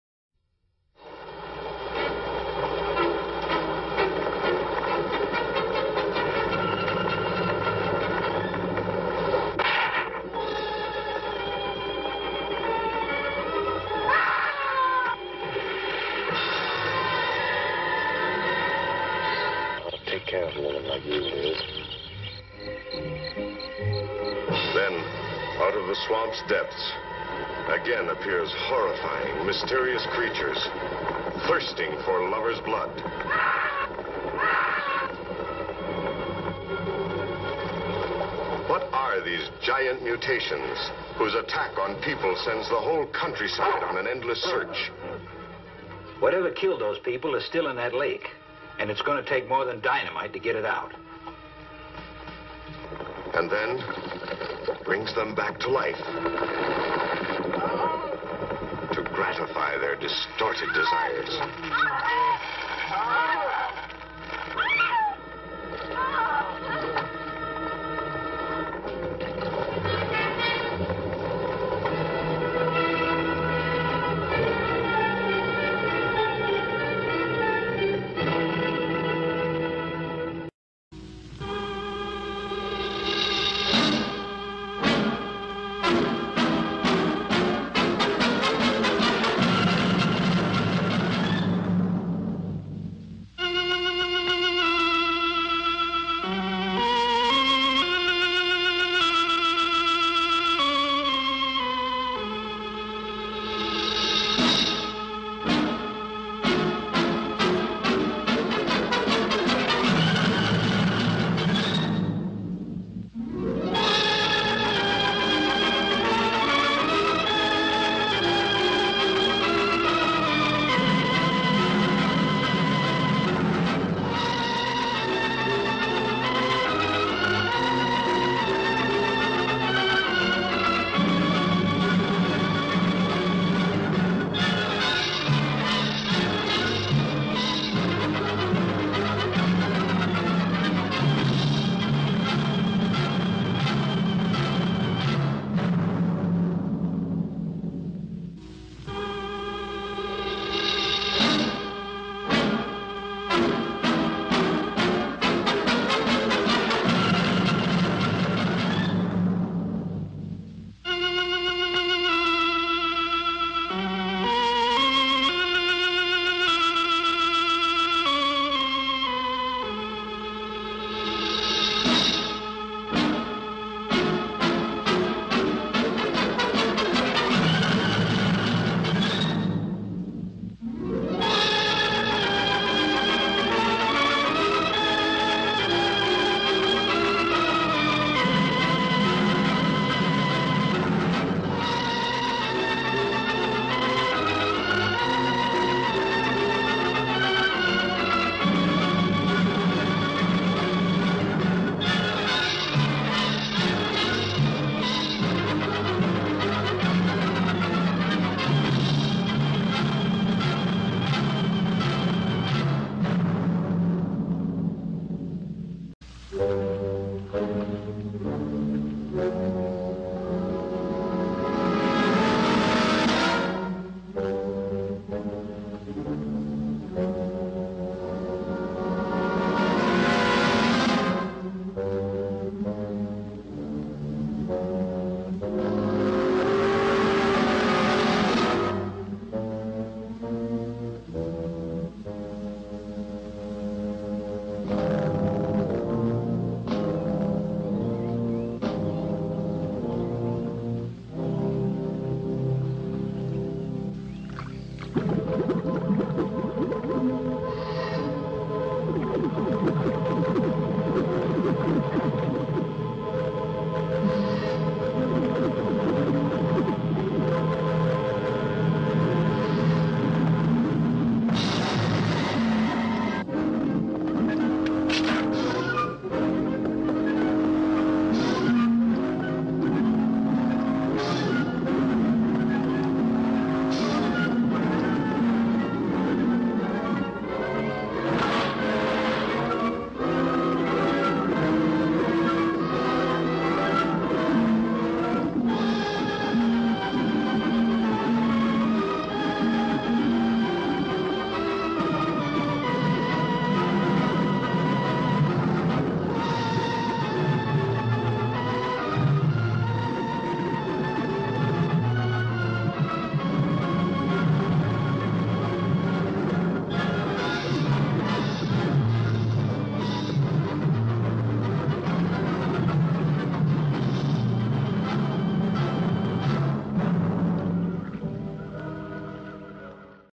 12inch Mutant Leech - Attack of the Giant Leeches 1959 Custom Figure
Another classic B-Movie character here friends, from the 1959 movie, Attack of the Giant Leeches. Roger Corman was executive producer for this movie. Video was filmed on location at Backford, check out the nifty swamp. had to film the figure there for some authenticity :) Enjoy.